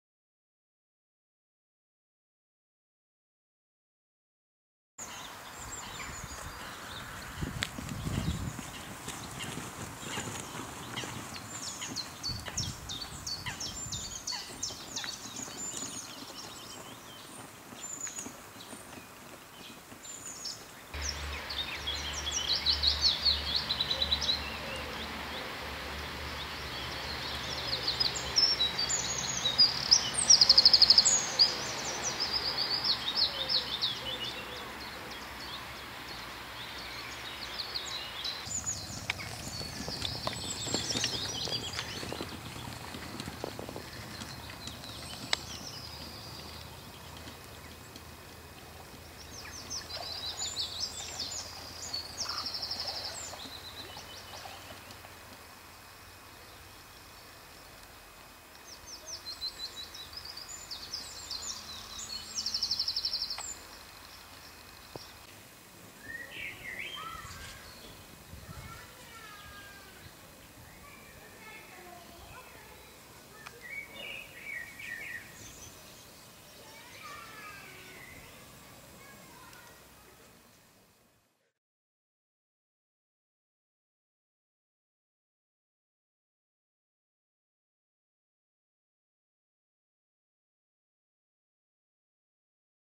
Walking Trailer Wheelie Pieterpad
The Wheelie Walking Trailer makes it possible to walk for greater distances without strain to the back and neck. Used for pilgrimages and expeditions but also for just everyday hiking trips.

In this video we walk part of the "Pieterpad" in Holland.